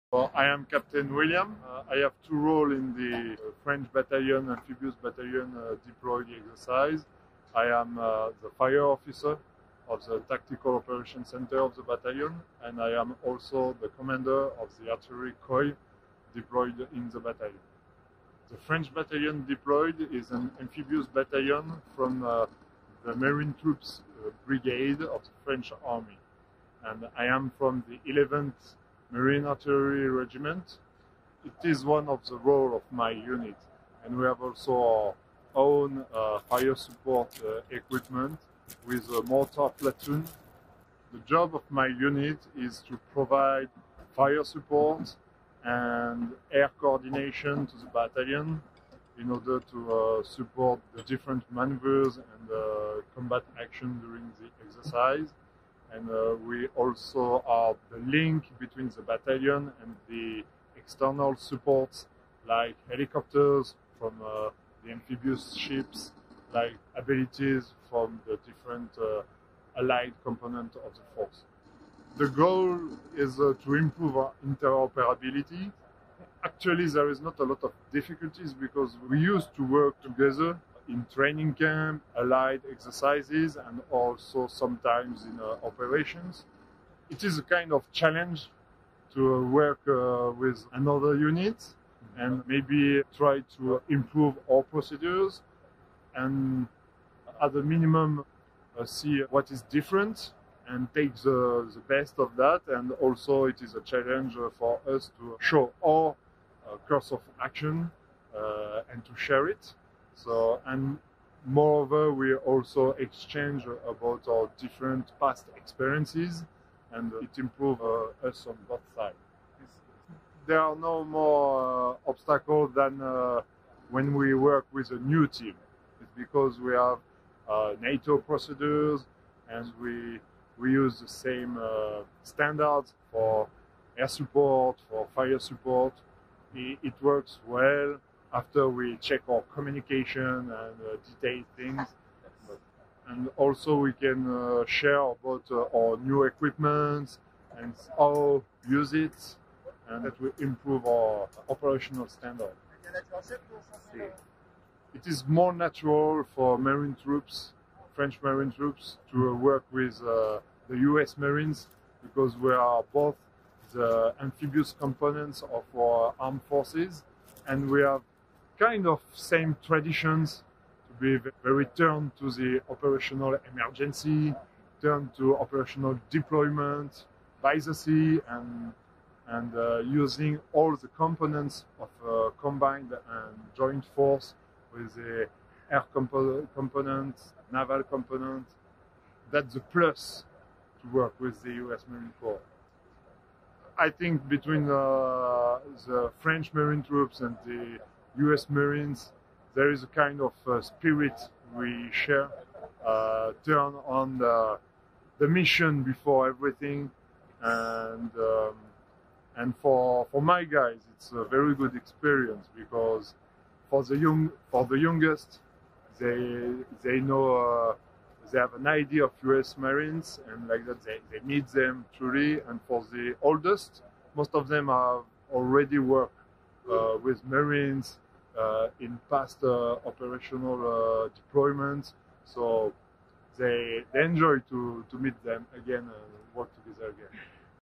2nd ANGLICO French Integration Interview Cold Response 2022
French Army Capt. William is interviewed about the integration of the French Army and U.S. Marines during exercise Cold Response 2022, Narvik, Norway, March 27, 2022.

2nd ANGLICO French Integration Interview Cold Response 2022
NARVIK, 18, NORWAY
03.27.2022

William is the commander of the 4th battery of the 11th Marine Artillery Regiment, and fire support coordinator of the French Onboard Battle Group. Exercise Cold Response '22 is a biennial Norwegian national readiness and defense exercise that takes place across Norway, with participation from each of its military services, as well as from 26 additional North Atlantic Treaty Organization allied nations and regional partners. (U.S. Marine Corps video by Cpl. Alexander Ransom)

Film Credits: Video by Cpl. Alexander Ransom